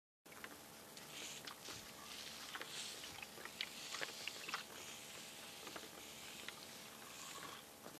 Abono de alga marina "SALGAX" en chile habanero
Aplicando abono de alga marina Salgax "CAMPO" en chile habanero
planta de 20 cm de altura